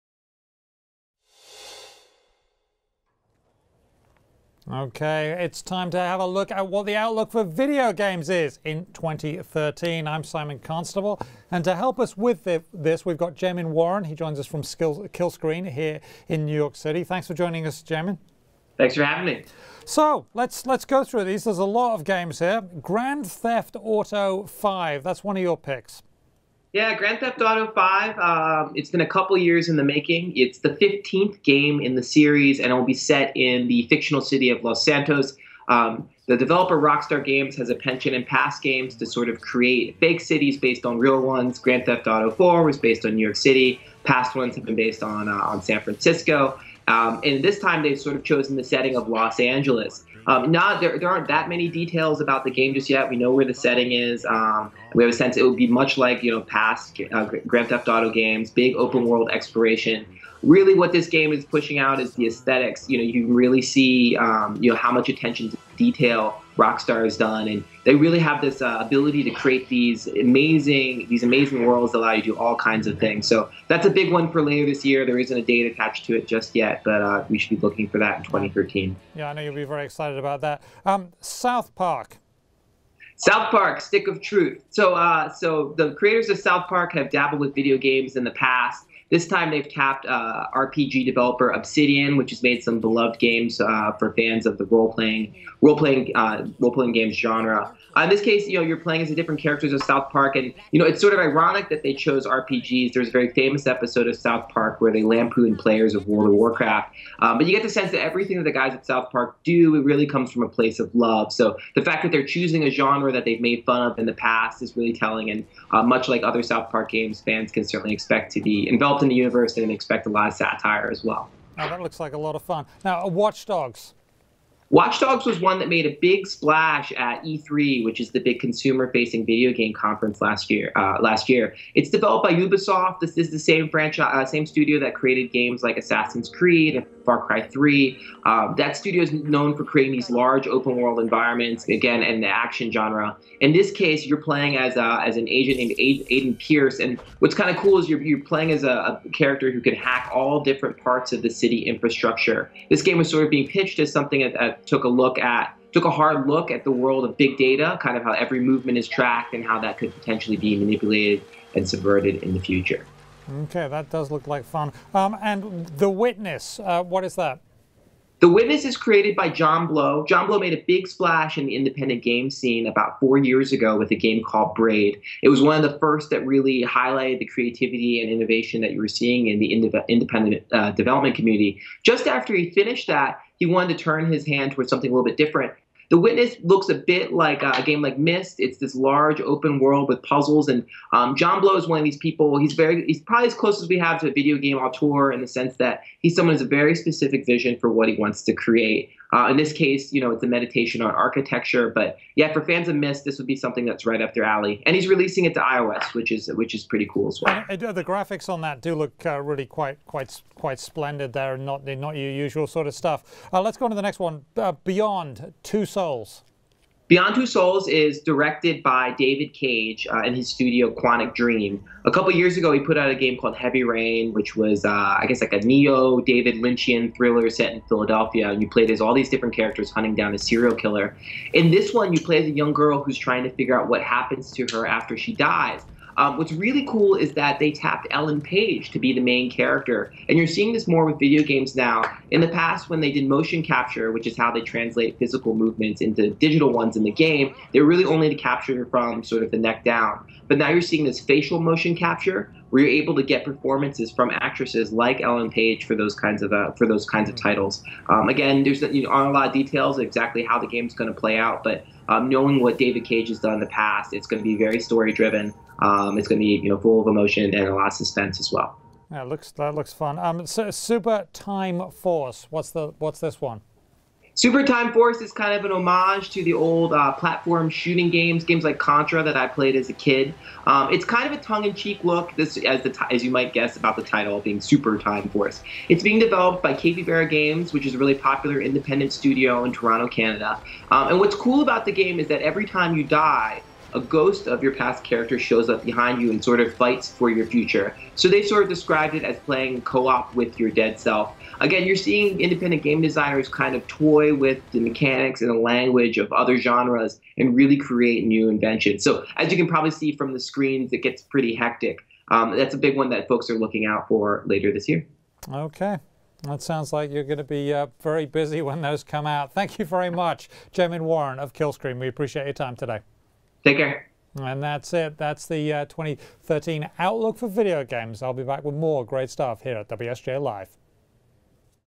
The Most-Anticipated Video Games of 2013
Kill Screen's Jamin Warren joins digits to preview 2013's most anticipated video game releases. Photo: Rockstar Games.